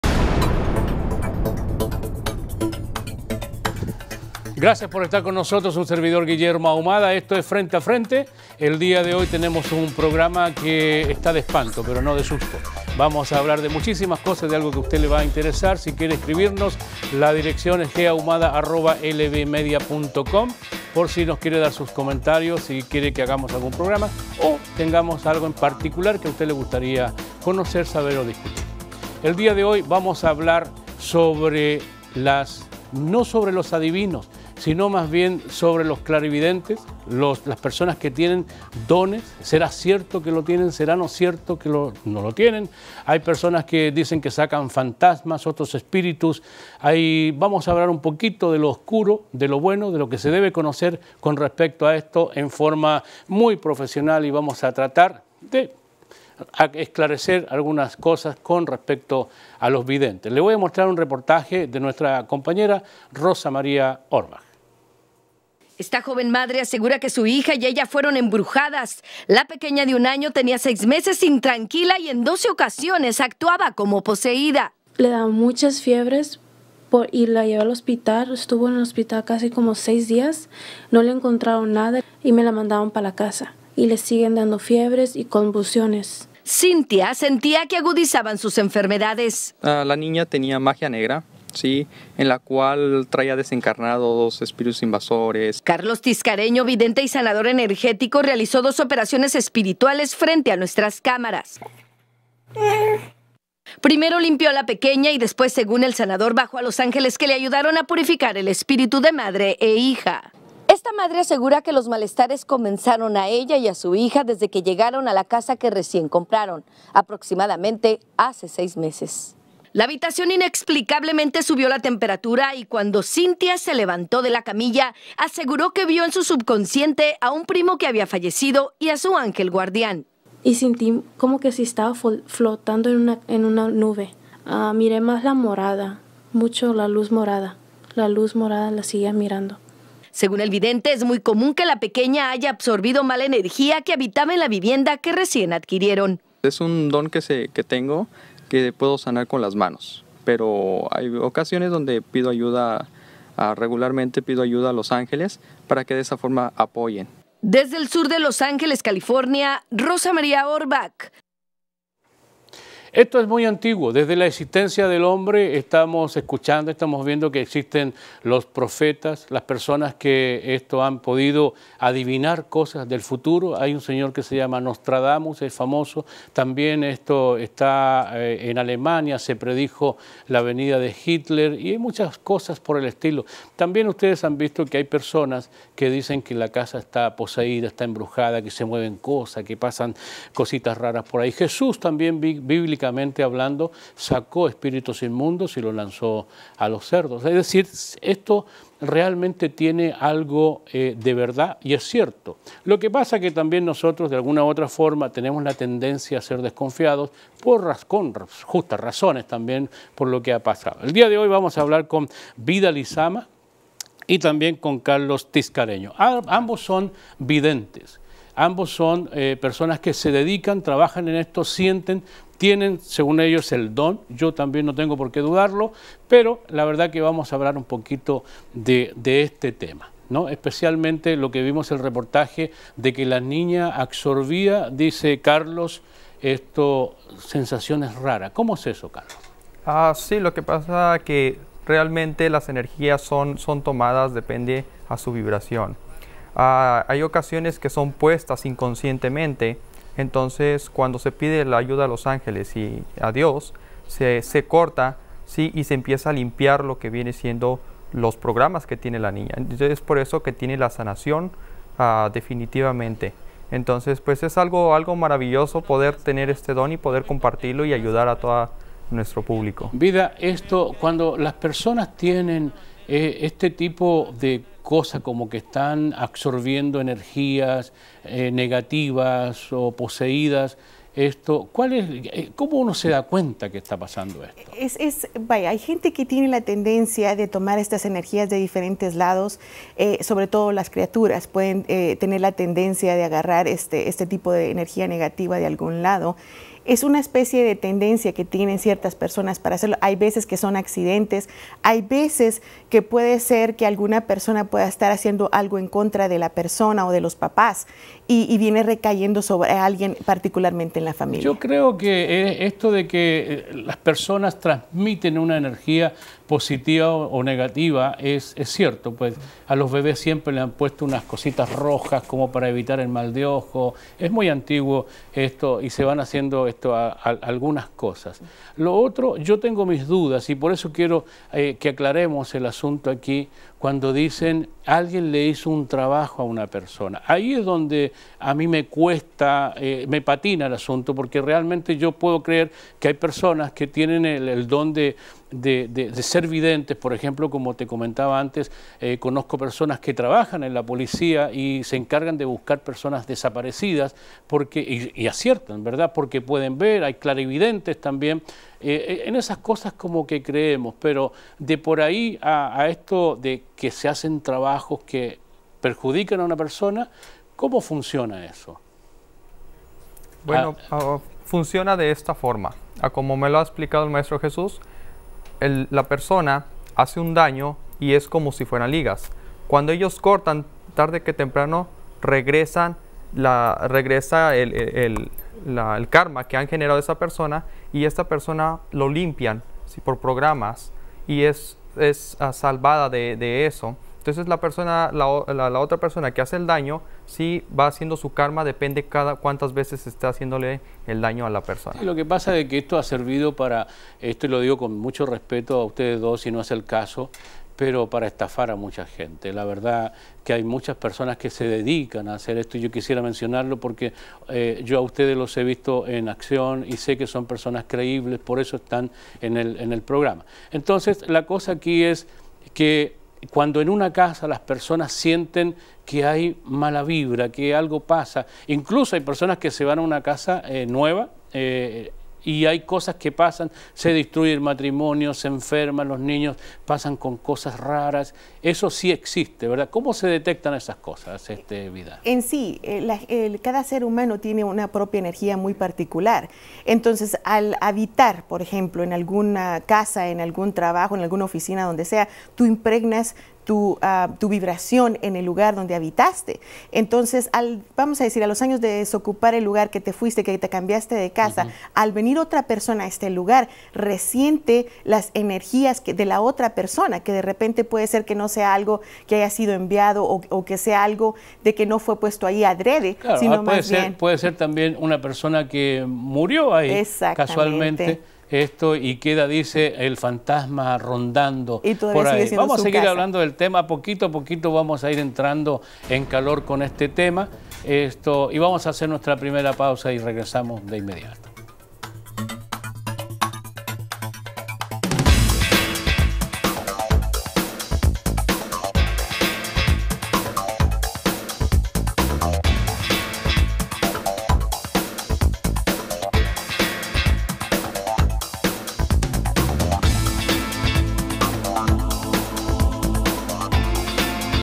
Videntes mitos y realidades 1
Un vidente o clarividente es la persona que pretende adivinar el porvenir o esclarecer lo que está oculto.
La clarividencia sería la capacidad hipotética de percepción extrasensorial por la cual algunas personas afirman recibir información por medios no-corporales y que escapan a los explicados científicamente por la física clásica. No existe ninguna evidencia científica adecuada de que tal capacidad sea existente, ni ningún experimento ha detectado fenómenos de clarividencia.